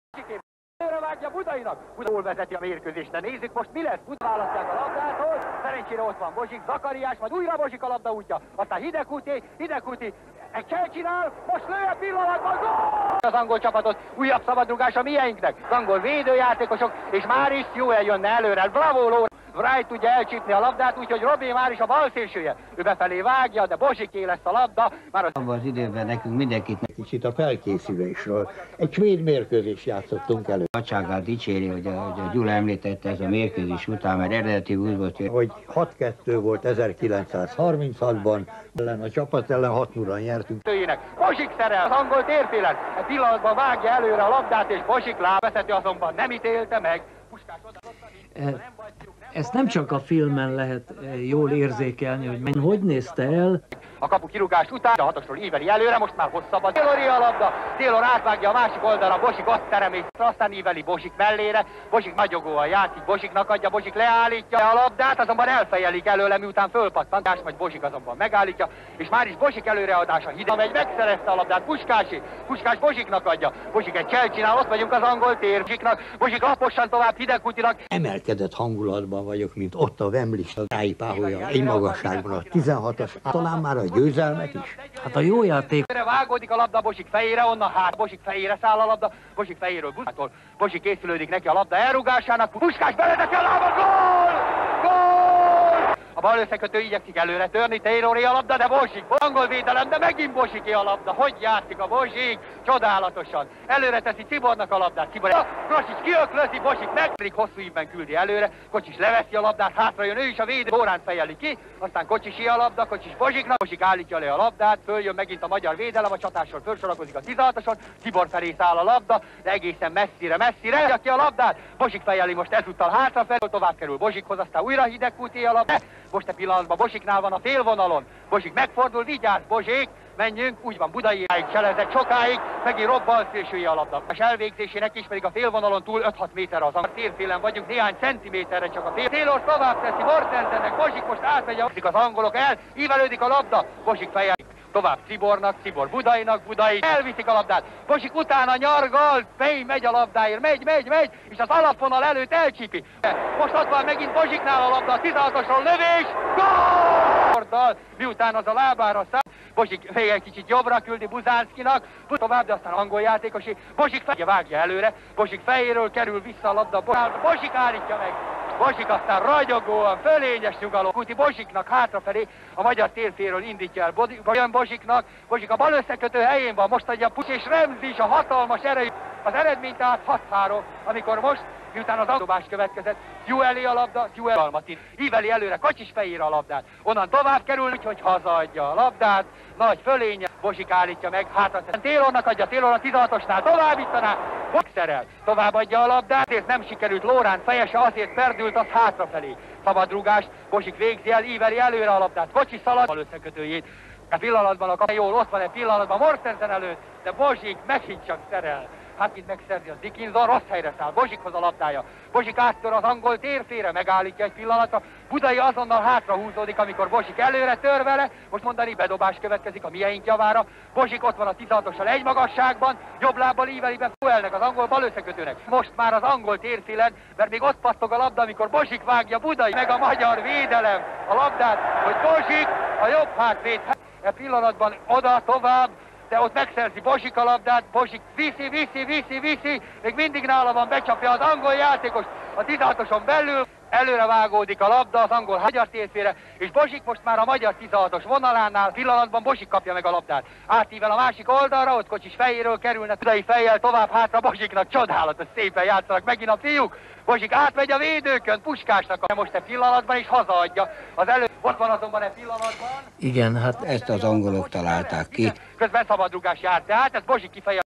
Jozsef Bozsik vs England (1953)
Touches of right-half Bozsik in the 'Match of the Century' at Wembley in 1953